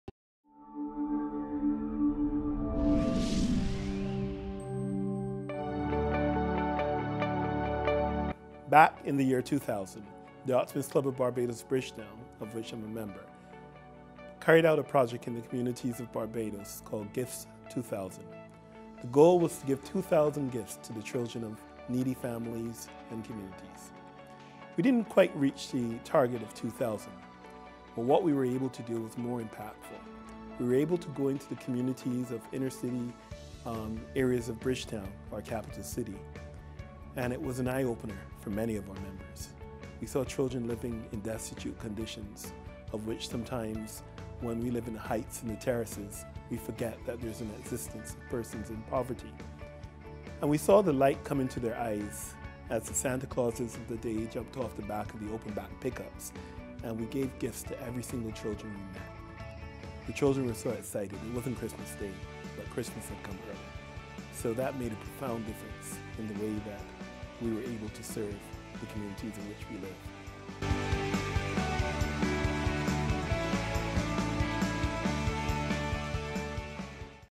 Optimist Story Adrian Elcock
In this week's OptimistStory, President-elect Adrian Elcock discusses the spirit of giving during the holidays.